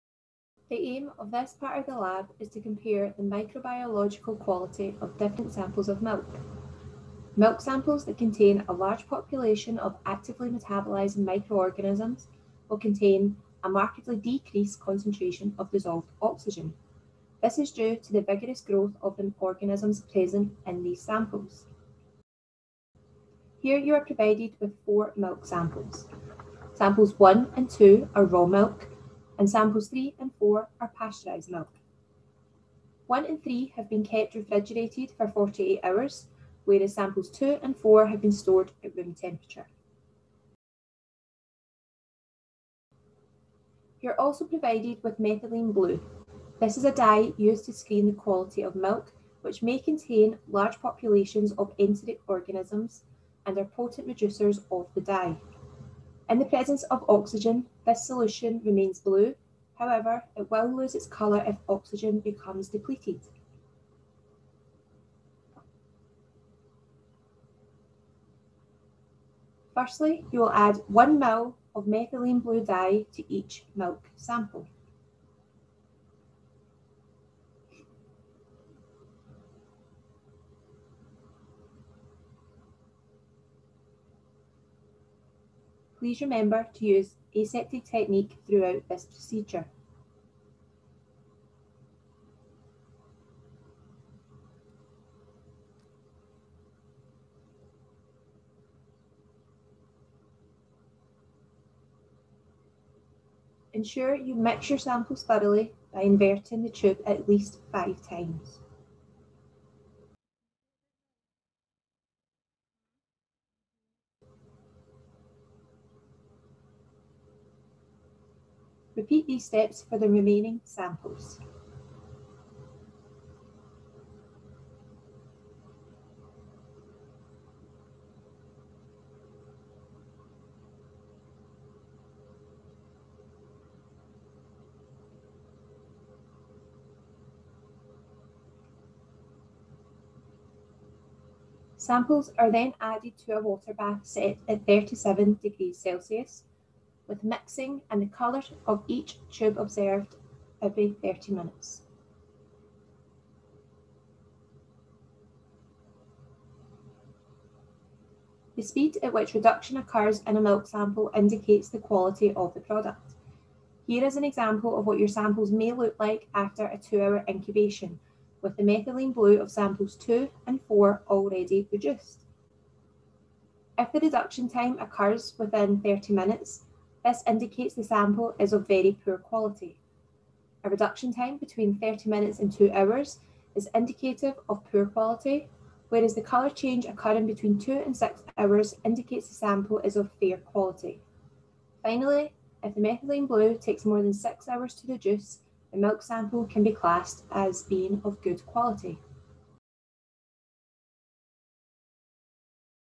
Level 3 Microbiology | Methylene blue reductase test
The methylene blue reductase test is designed to screen the quality of milk, that may contain large populations of enteric organisms which are potent reducers of the dye. The speed at which reduction occurs following the addition of methylene blue to a milk sample indicates the milk’s quality.